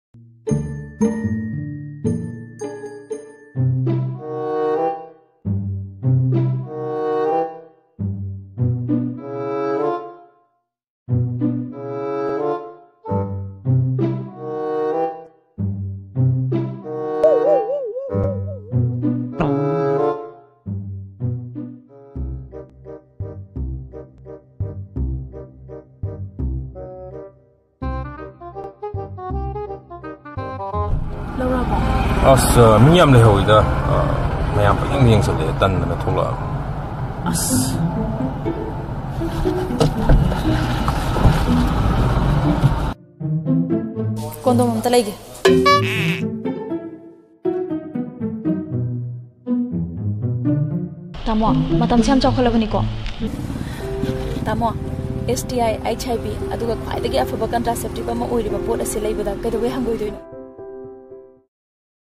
Use CONDOM to protect from HIVAIDS
Condom Promotion is about importance of using Condom

Condom can protect from
1.HIV
2.STI (Sextually Transmitted Infections)
3.Unwanted Pregnancy